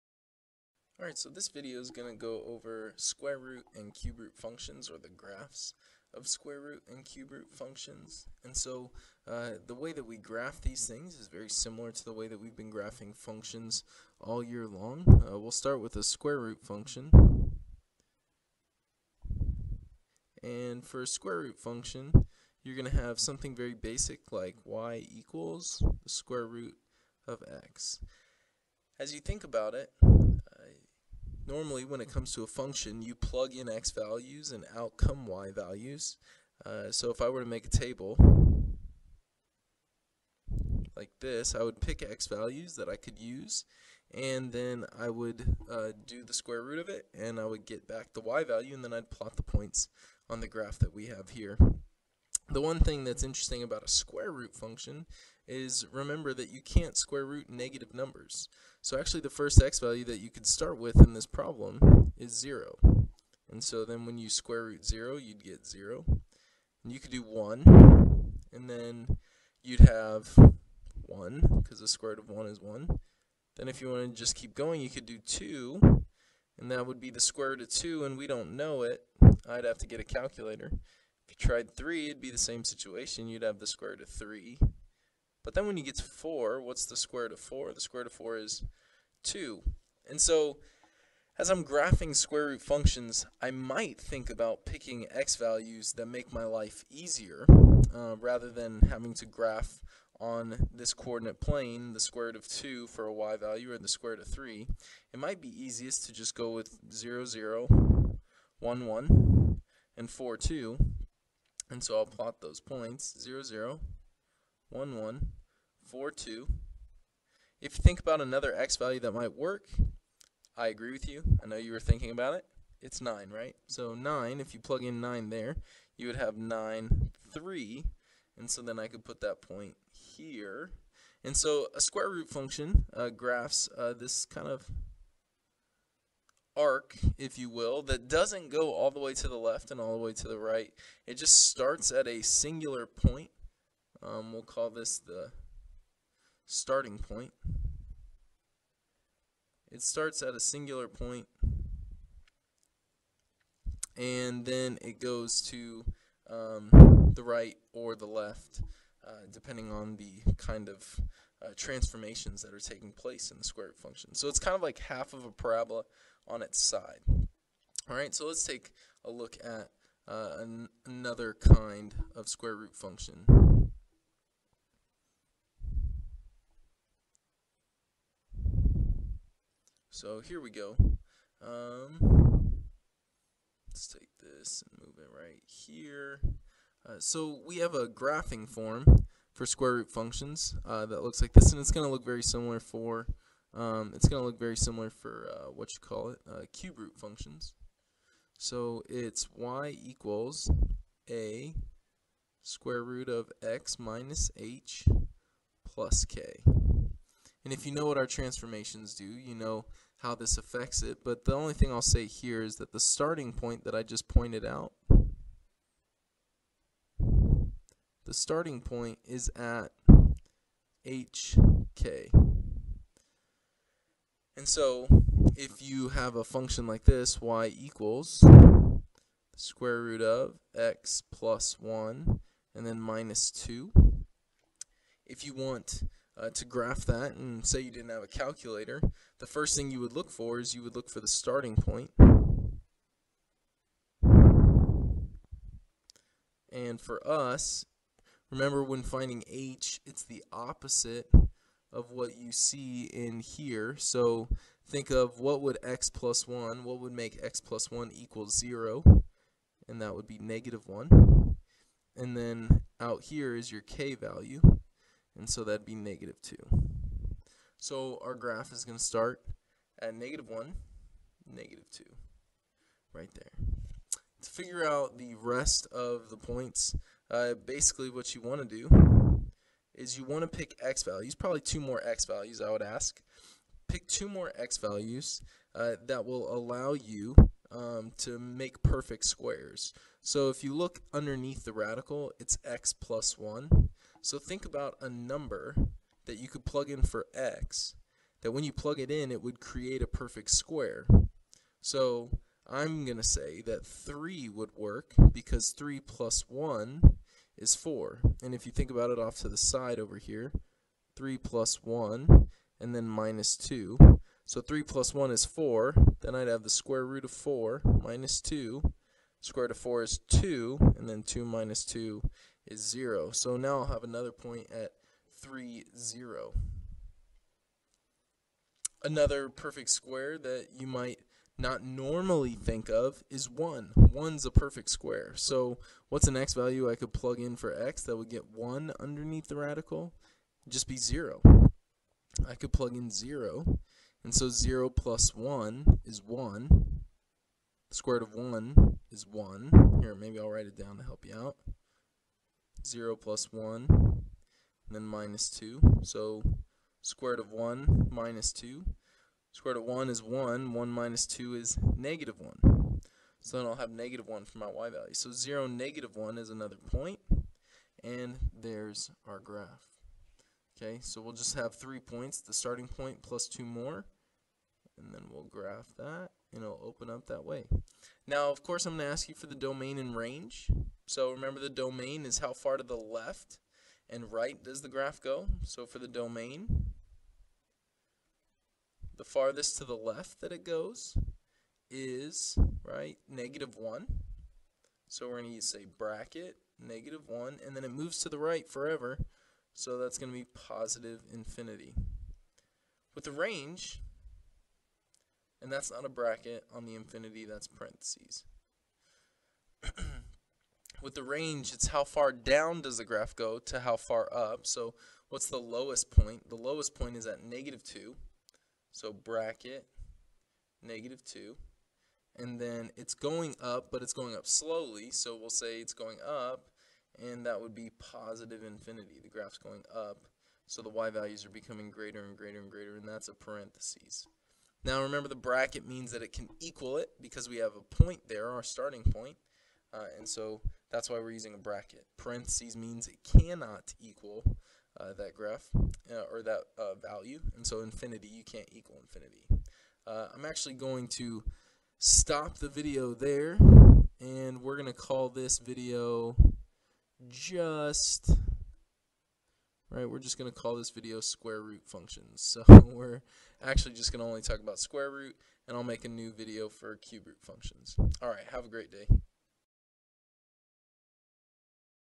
Graphing square root functions
This video briefly explains how to graph a square root function.